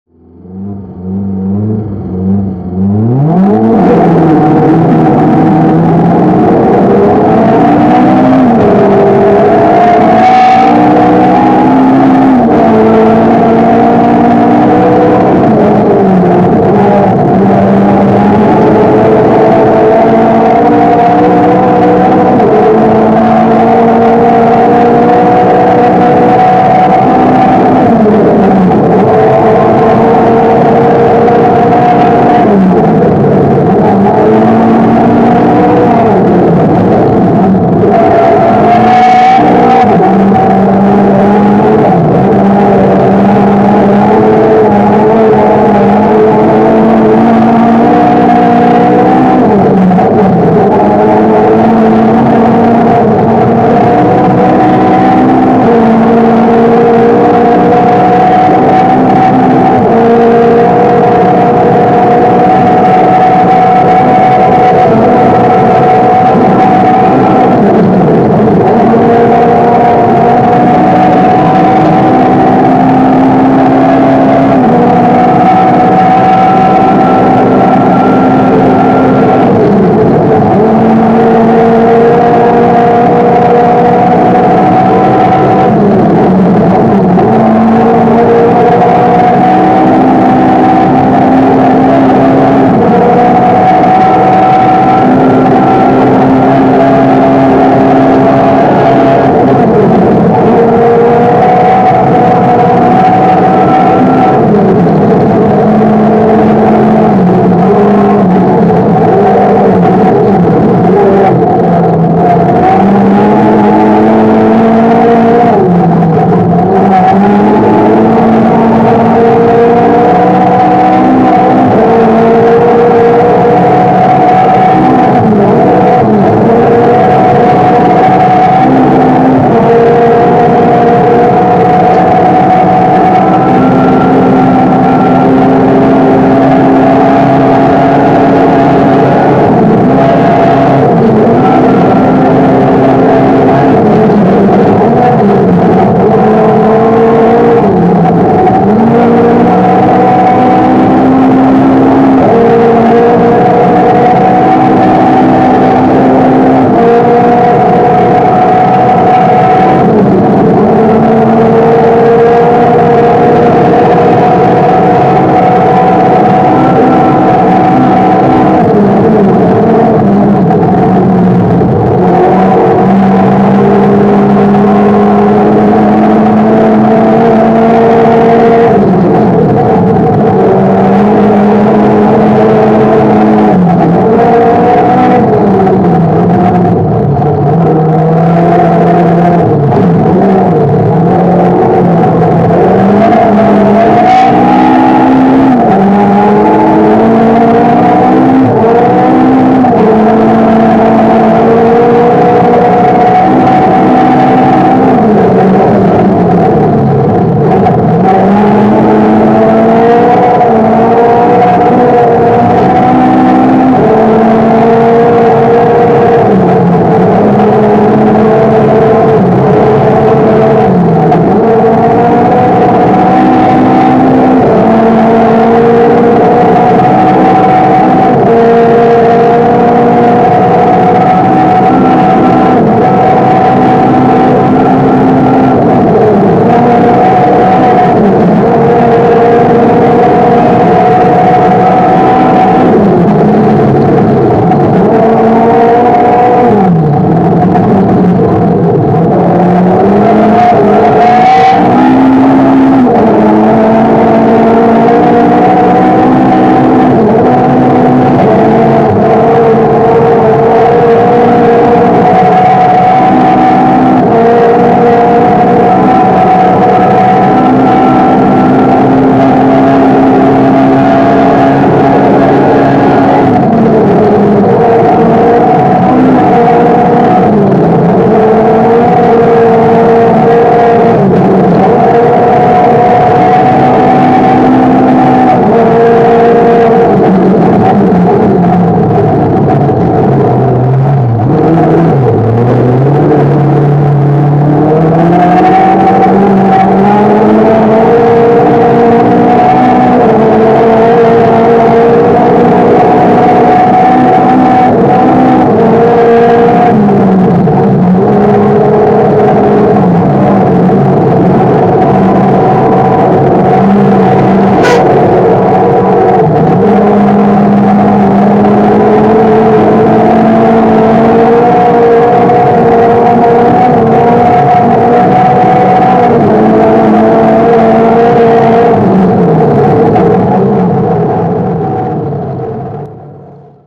Piippola ralli 12.1.2019 EK3
Nro. 218 Ford Focus

Eturenkaista alkoi olla piikit vähissä pätkän lopulla, niin vähän oli jo liukasta.